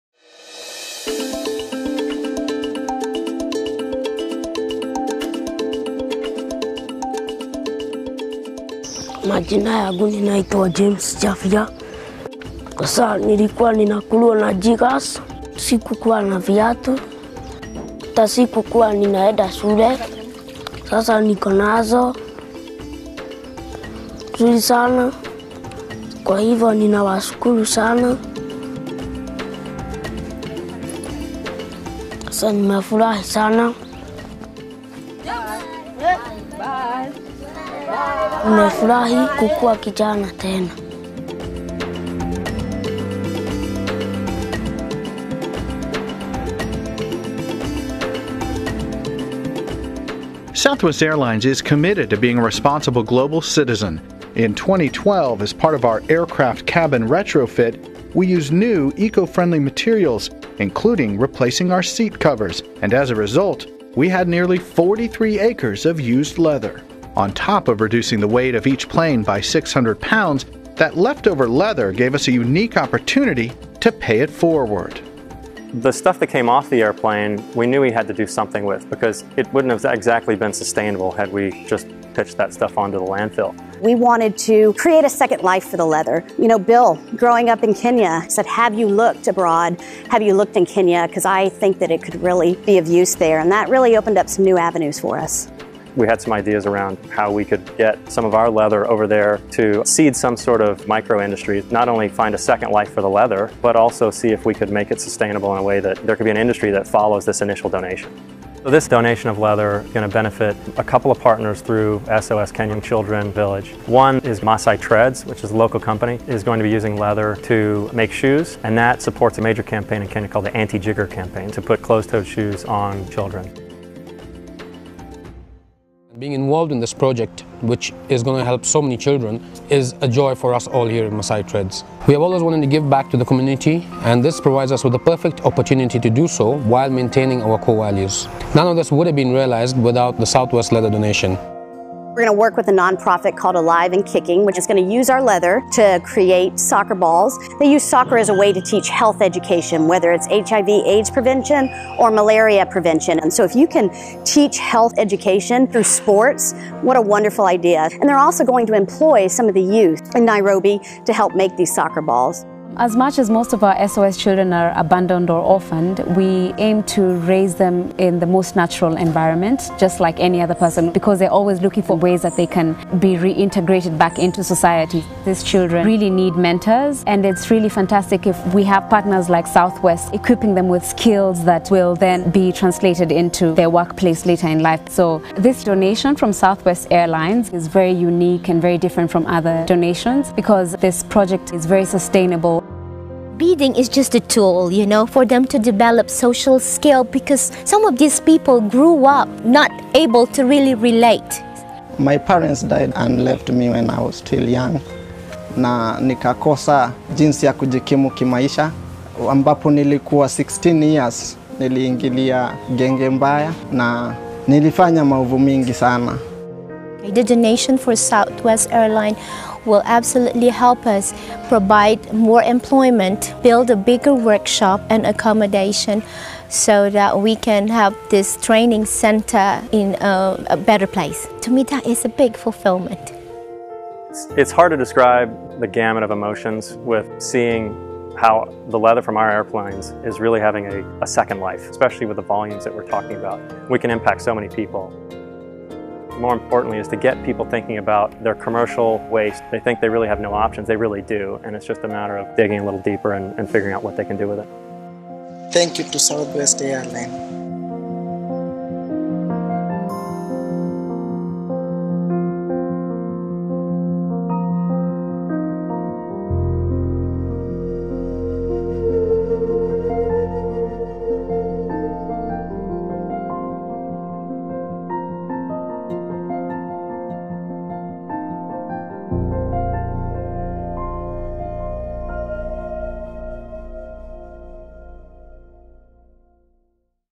Southwest Airlines Launches Luv Seat: Repurpose With Purpose
Southwest Airlines (NYSE: LUV) announced today the launch of LUV Seat: Repurpose with Purpose, a global sustainability initiative to upcycle 43 acres of used leather seat coverings into new products that will benefit communities by providing employment, skills training, and donated products.  or visit Nuts About Southwest to read the blog post here.

LUV Seat: Repurpose with Purpose was developed following the Company's Evolve program, a large-scale redesign of all 737-700 aircraft, a portion of its 737-300 fleet, and is now standard on all new planes. The Evolve program replaced the leather seat covers and other interior elements with environmentally friendly materials.  Through this redesign, Southwest reduced the weight of each aircraft by more than 600 pounds.

LUV Seat is an innovative upcycling project born from the Evolve program. By donating the used leather for social impact and keeping it out of landfills, Southwest Airlines is reinforcing its commitment to long-term global sustainable business operations and positioning itself as a leader in corporate responsibility.